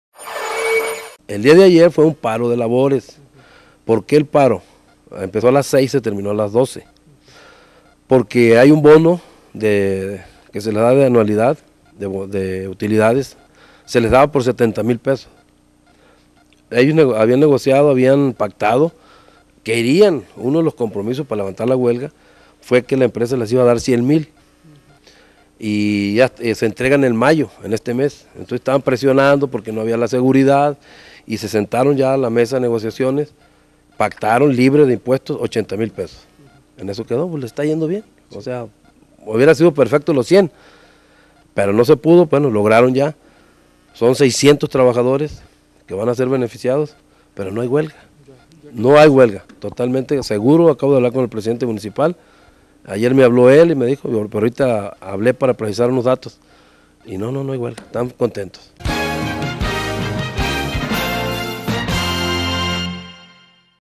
JAIME RIVAS LOAISA DIP LOCAL PRI
Quedo solucionado el paro de labores de la Compañía Minera de Tayoltita, así lo da a conocer el diputado Jaime Rivas Loaiza.